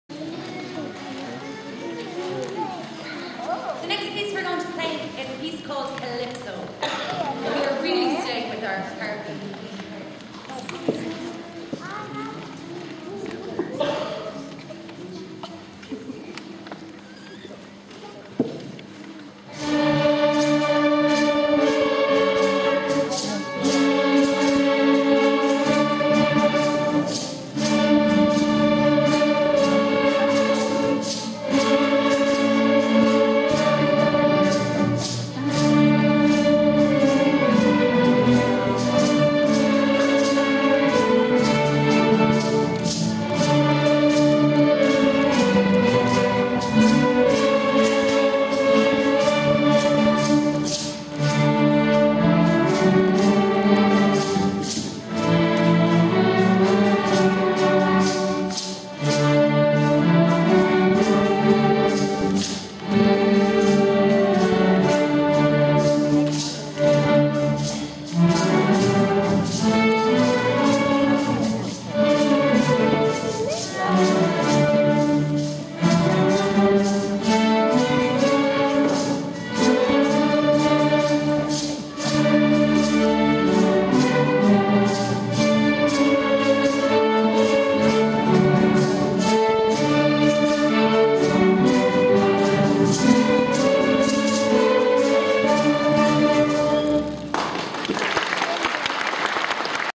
Sounds Ensemble St Agnes Hall 260211-2
Sounds Ensemble (black shirts) join forces with St Agnes's Parents Orchestra (red shirts)  to present a concert in aid of Concern's Haiti Appeal , 26th Feb 2011.